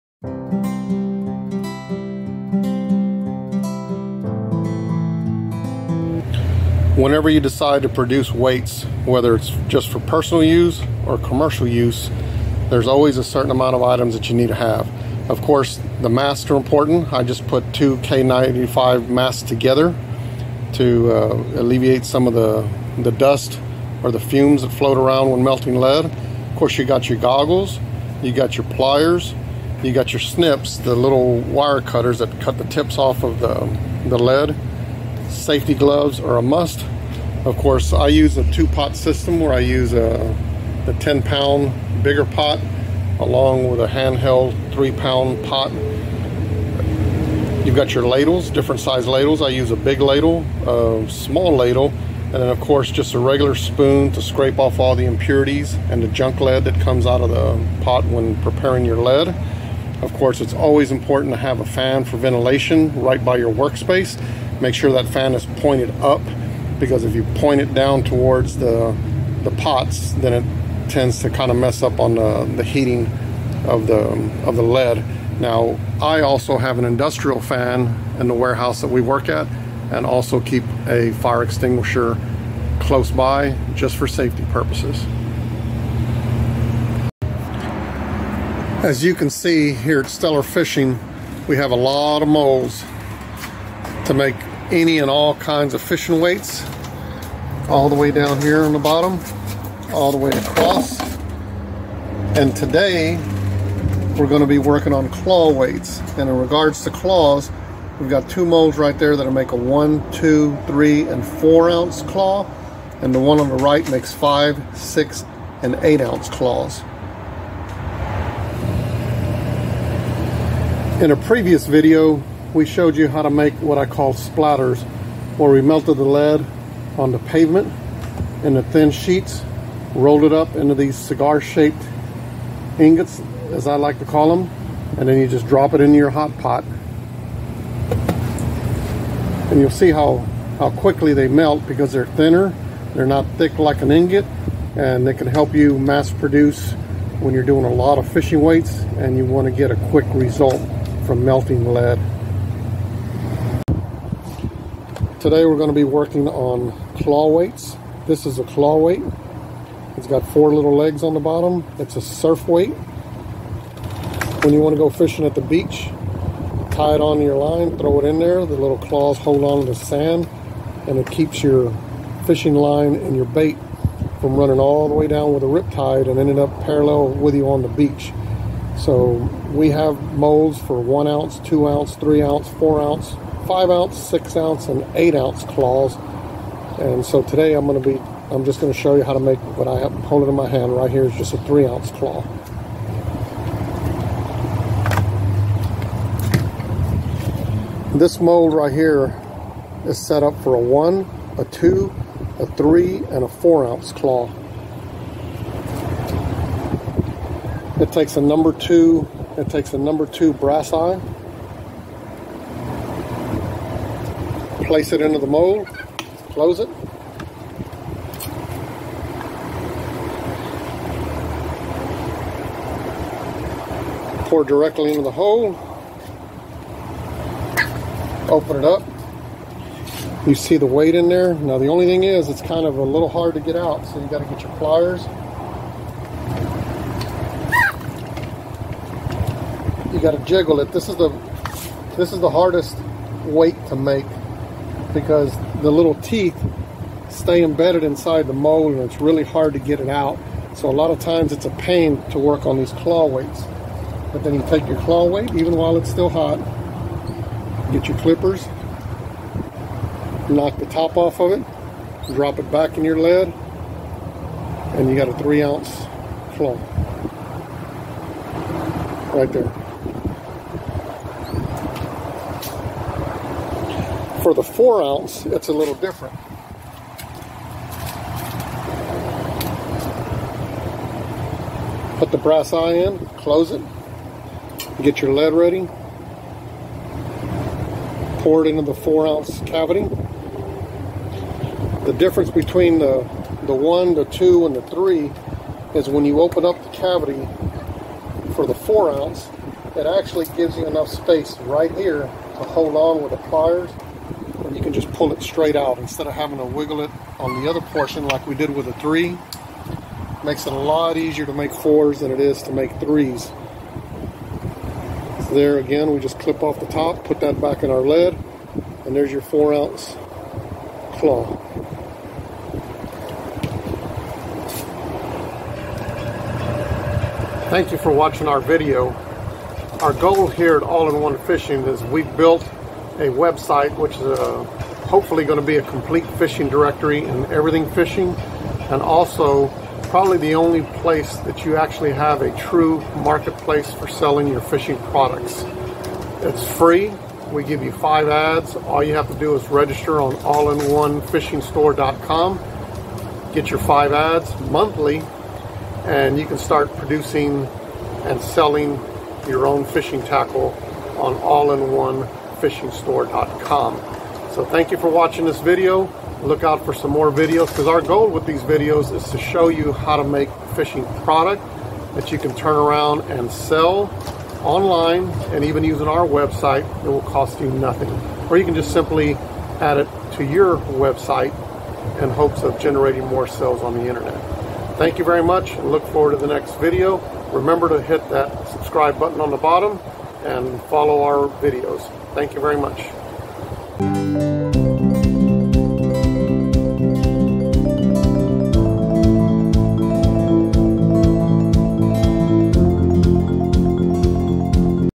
How To Make Claw Fishing Weights EASY!
This video will explain how to make claw shaped fishing weights.